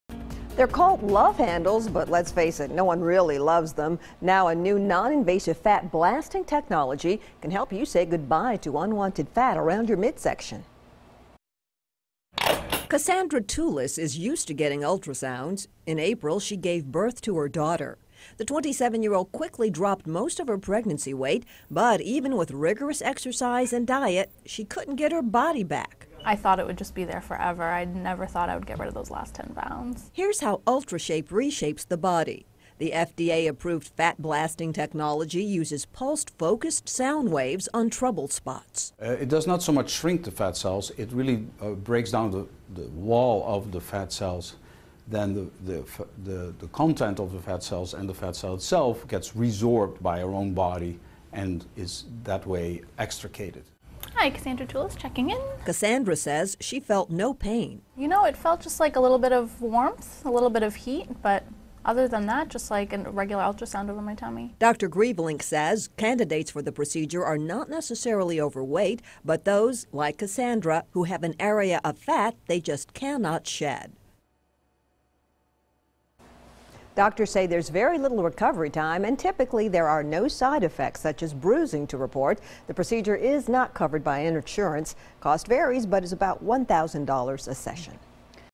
Winning The Battle Of The Bulge
They’re called love handles but let’s face it, no one really loves them. Now, a new non-invasive fat blasting technology can help you say goodbye to unwanted fat around your midsection.